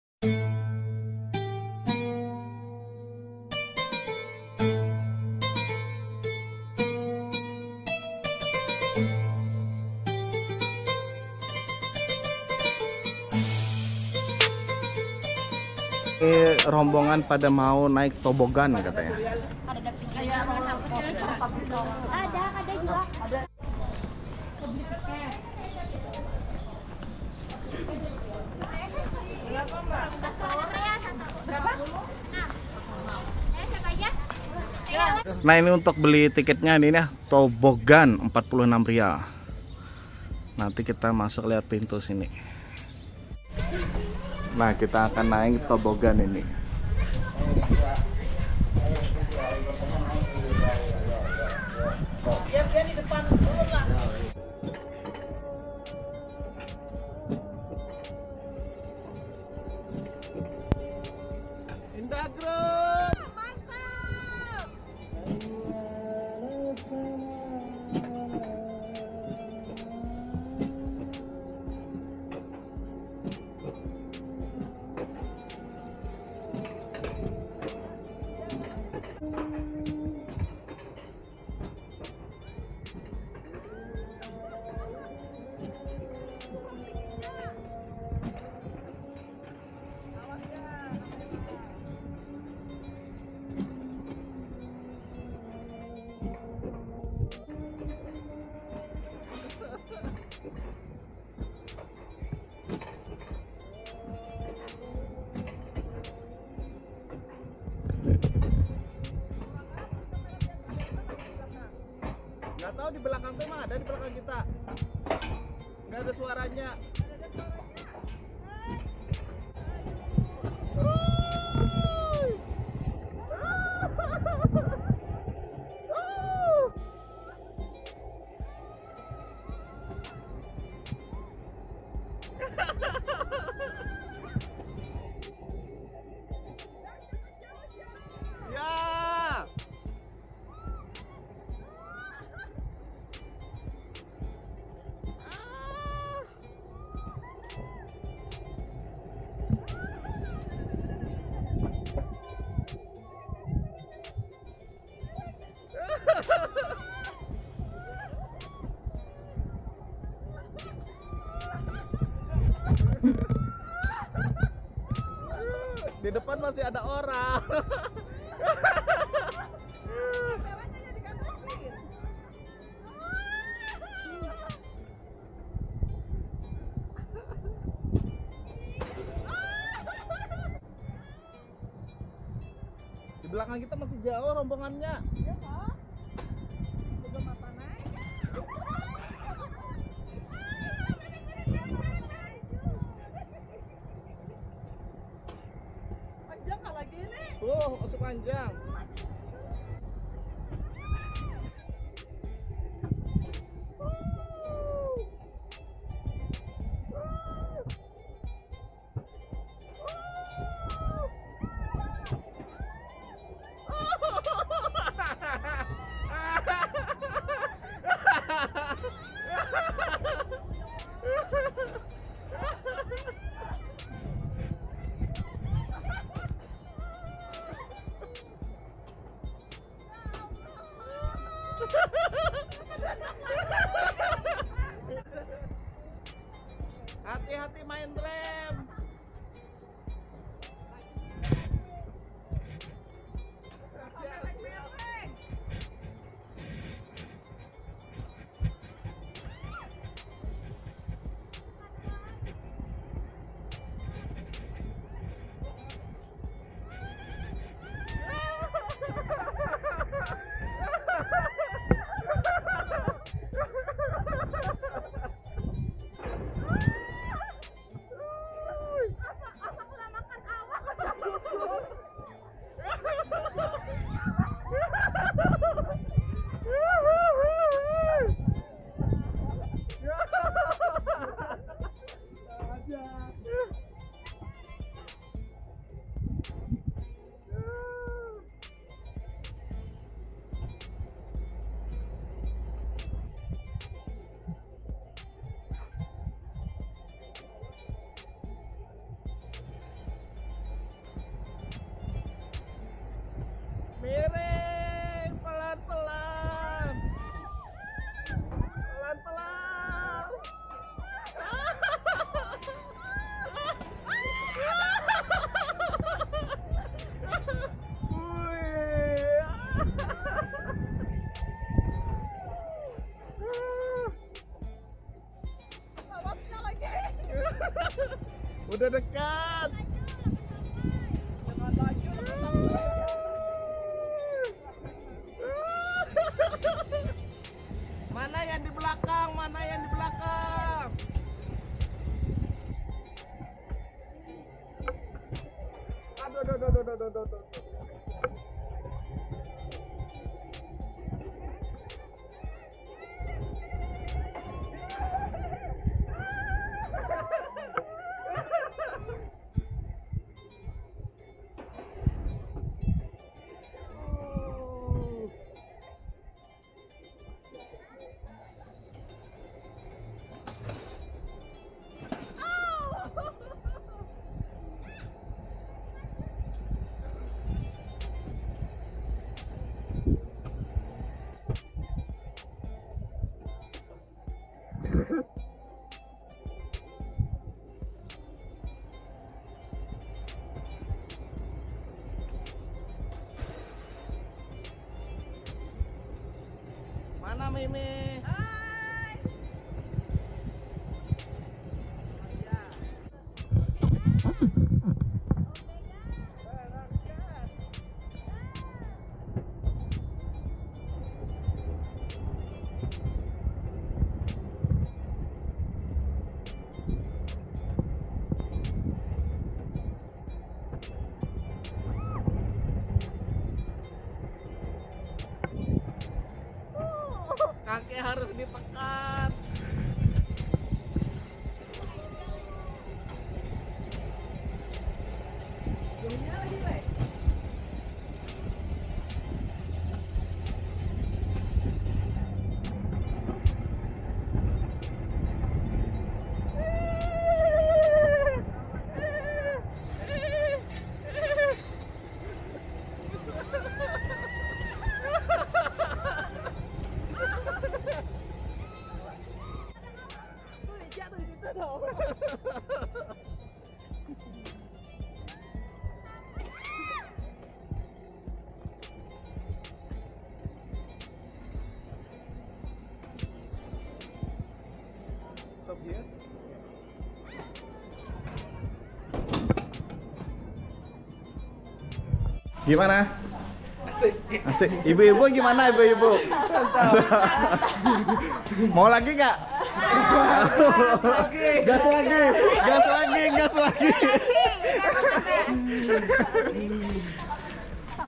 #40 Begini Keseruan Naik Toboggan di Thaif... Berani Coba??
Jangan lupa semua rangkaian video Umroh Plus Turki 2023 kami 😊

Terimakasih 😊😊🙏🙏